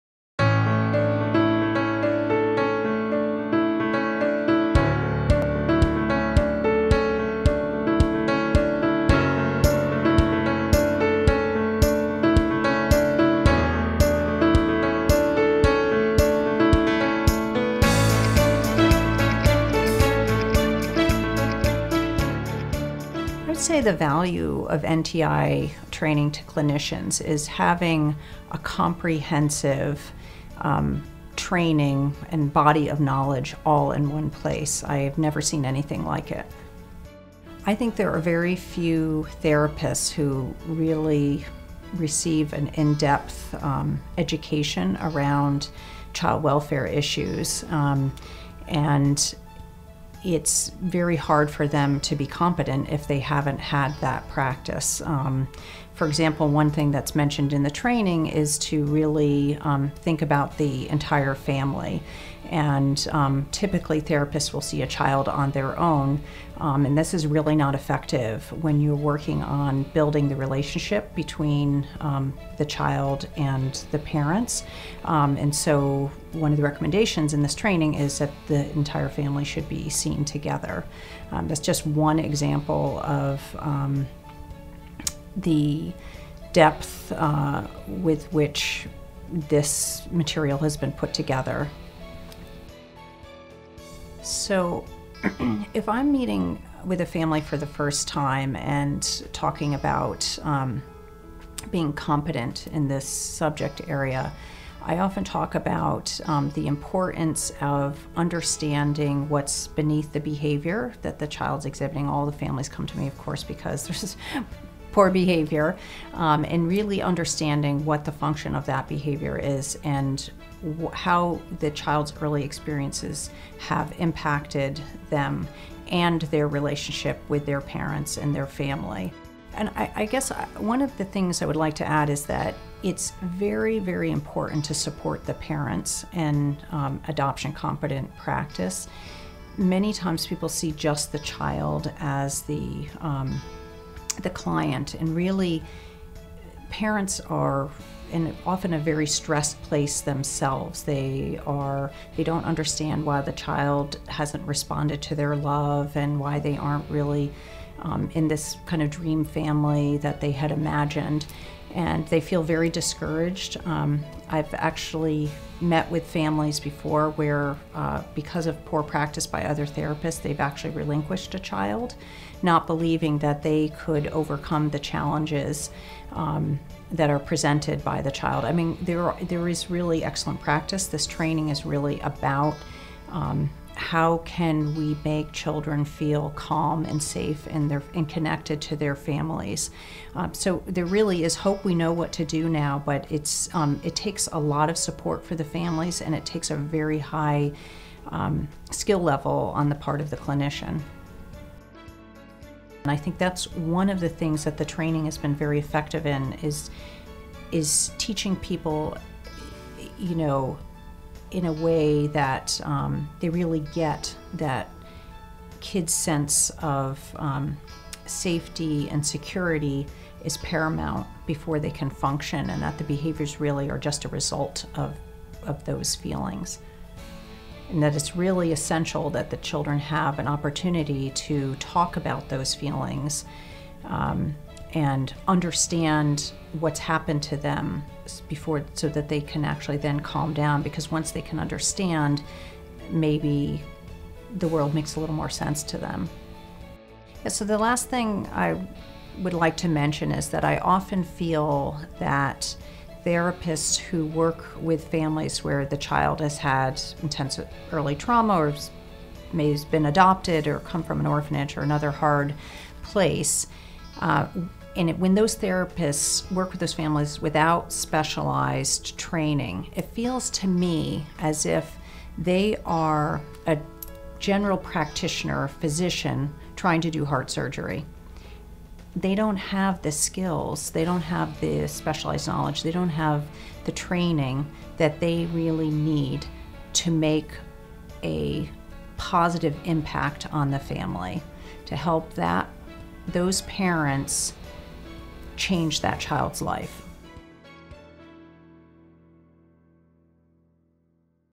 The Value of NTI for Therapists
Sarah Gerstenzang, Board Chair of the NY Adoptive and Foster Family Coalition, and an Adoptive Parent, explains why therapists must have the skills and knowledge that NTI Training helps build.